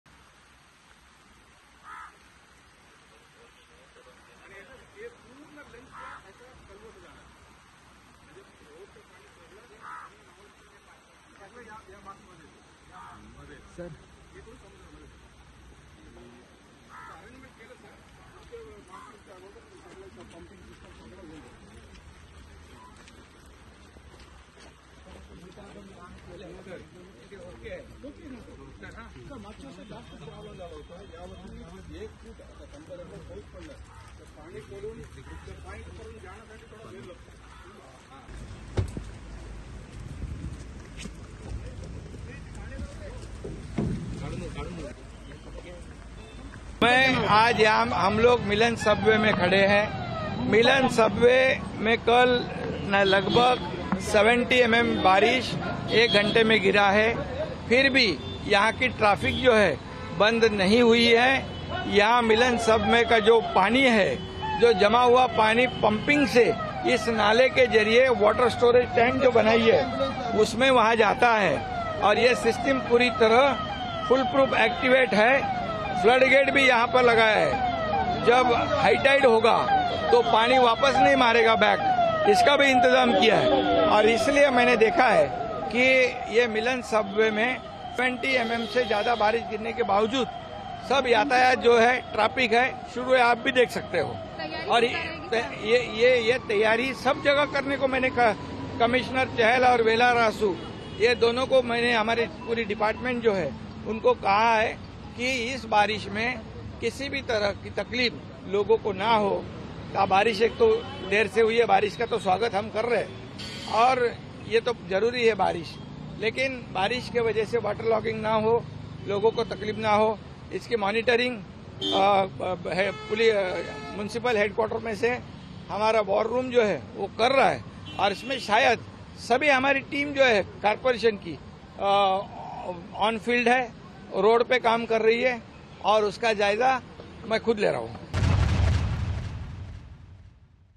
Mumbai: भारी बारिश के बीच CM Eknath Shinde ने तटीय इलाकों का जायजा लिया | Maharashtra | Monsoon
भारी बारिश के बीच महाराष्ट्र के मुख्यमंत्री एकनाथ शिंदे ने मुंबई वर्ली के तटीय इलाकों का दौरा कर स्थिति का जायजा लिया।मानसून आने के साथ ही शहर में भारी बारिश के कारण कई इलाकों में जलभराव हो गया है।आईएमडी ने सोमवार और मंगलवार के लिए येलो अलर्ट जारी किया है।